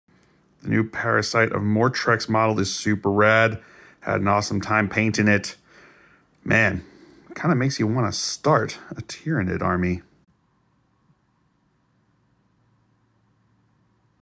Painted the New Tyranid Model! ...
Games Workshop was kind enough to send over the New Tyranids Codex and the new Parasite of Mortrex model early! I painted up the new model and filmed a Batrep that will be coming out on Monday!